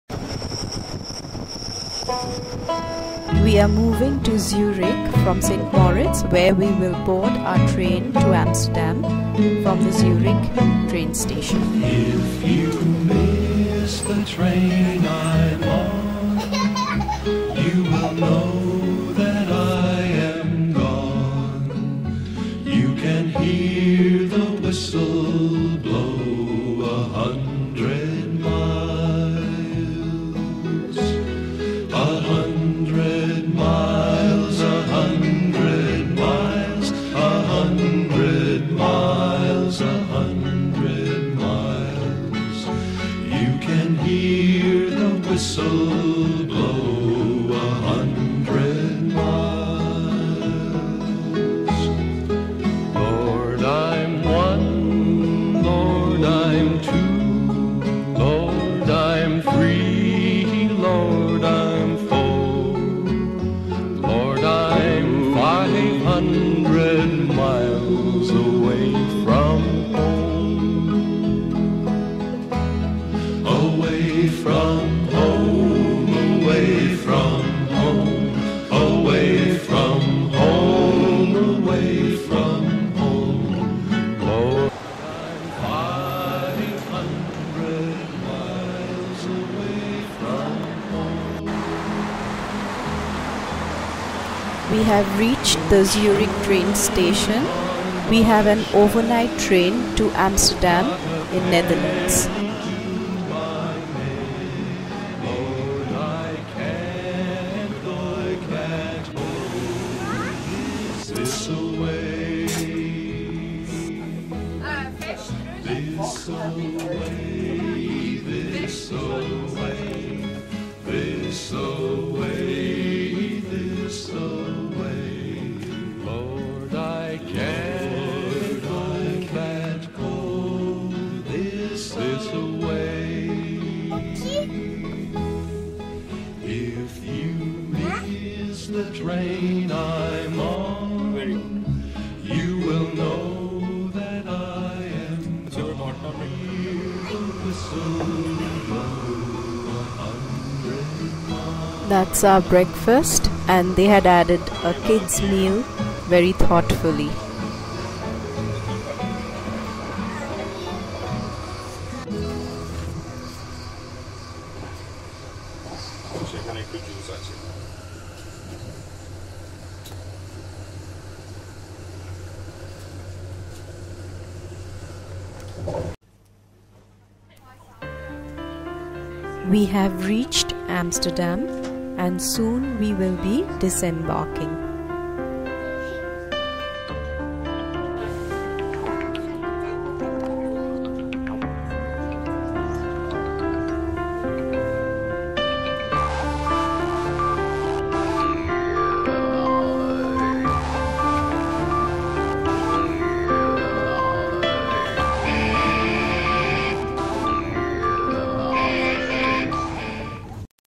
Train from Zurich to Amsterdam 2010
we move onto our next destination Amsterdam, Netherlands. Our journey from St.Moritz to Zurich Train Station and then an overnight train to Amsterdam.